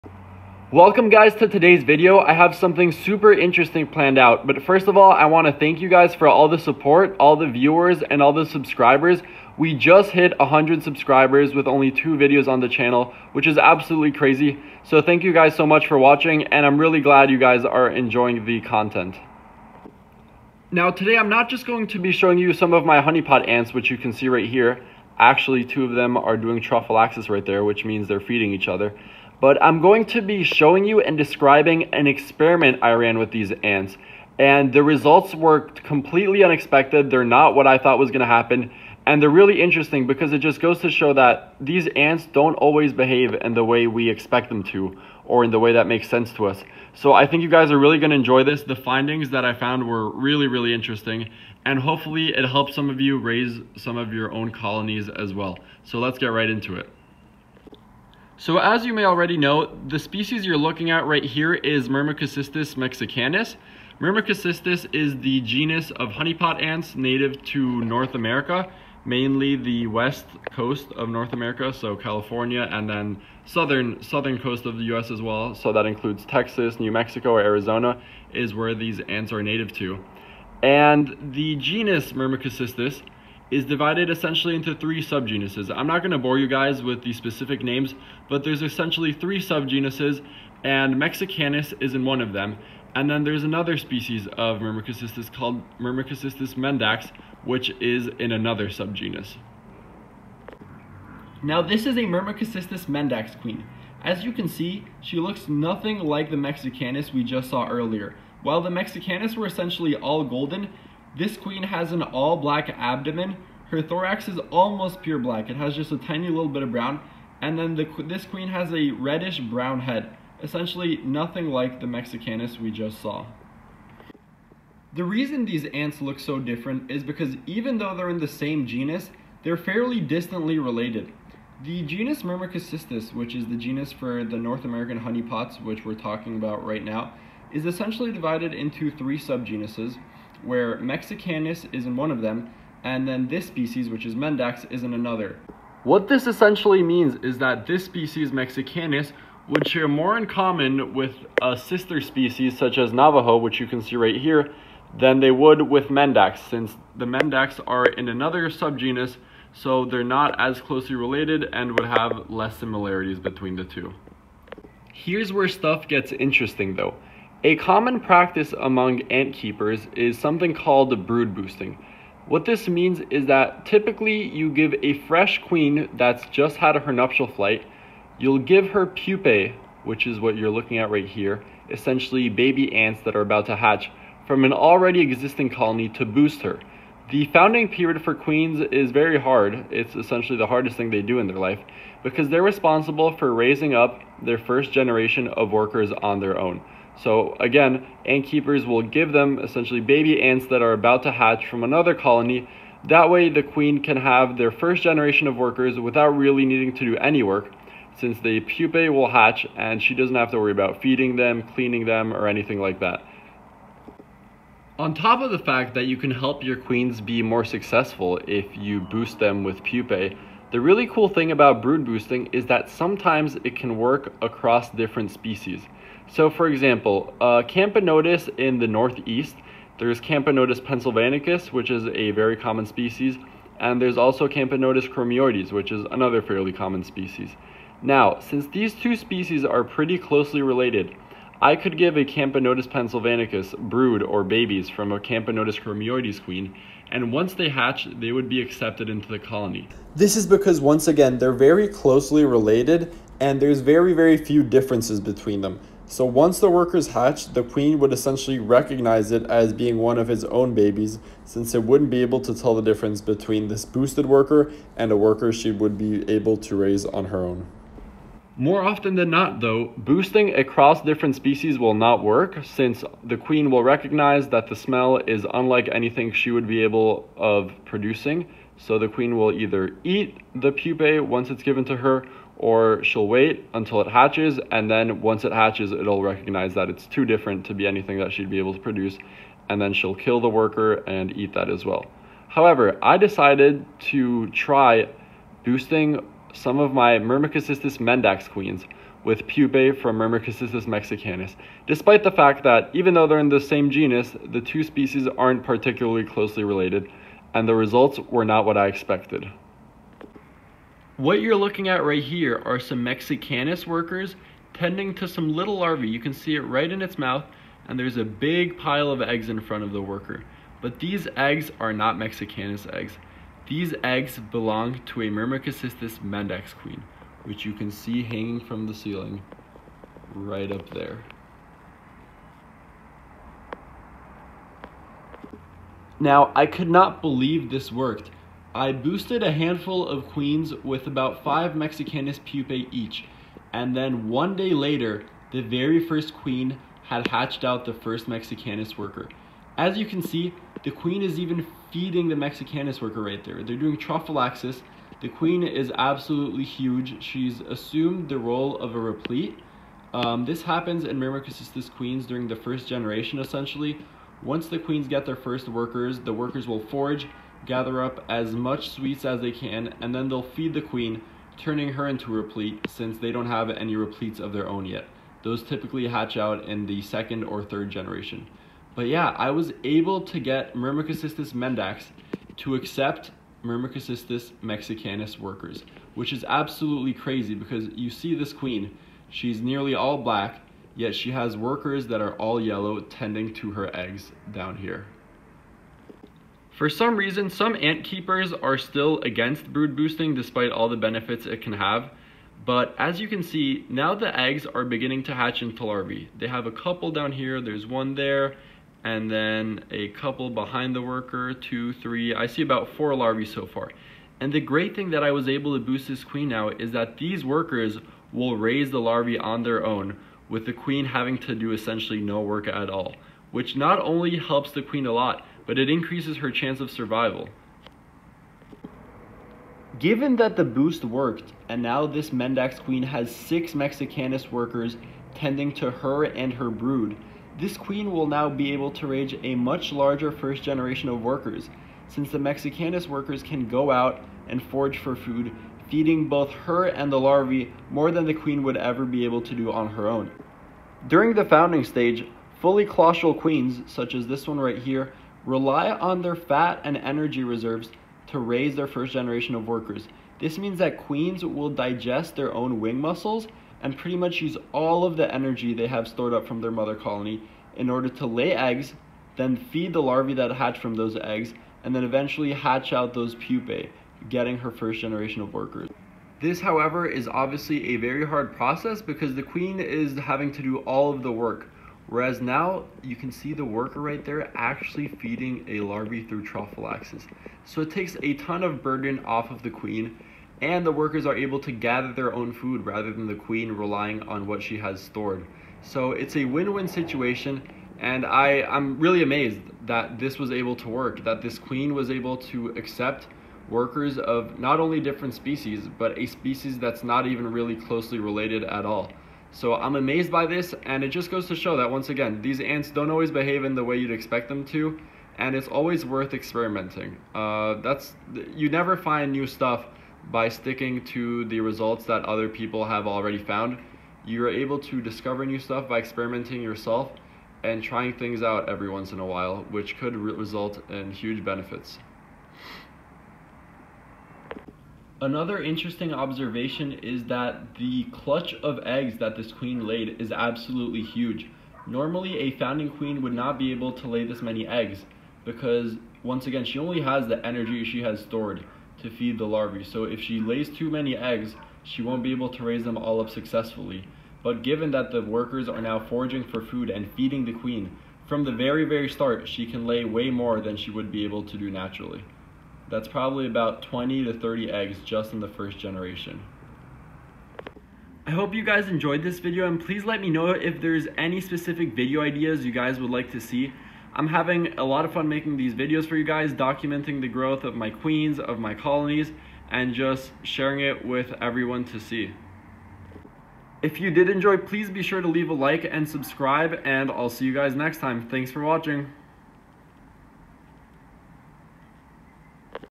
CRAZY ANT EXPERIMENT COMBINING DIFFERENT SPECIES! Myrmecocystus mendax and mexicanus!
Today I'm showing you one of my Myrmecocystus mexicanus colonies and a crazy experiment I decided to do with some fresh Myrmecocystus mendax queens I have. The results are unbelievable!